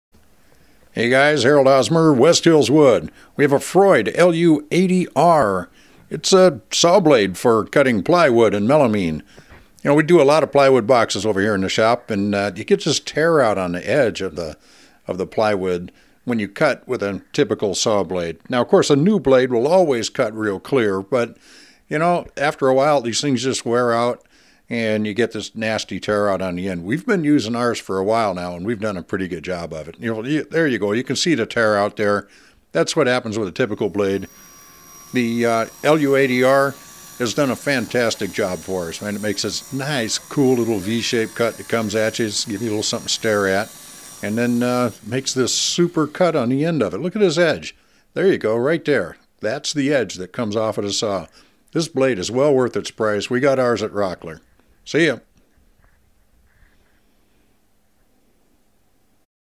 DIY Freud LU80R Table Saw Plywood Blade Review
We make a lot of plywood boxes in our hobby shop and have gone through multiple saw blades along the way. Then we splurged and bought a Freud LU80R at the Rockler in Torrance.

I forget what we paid for it, but this thing is outstanding in every way. Super clean edges on all our cuts, longest blade we've ever had, and the only drawback is that it's super sharp and we've nicked ourselves a time or two when installing it.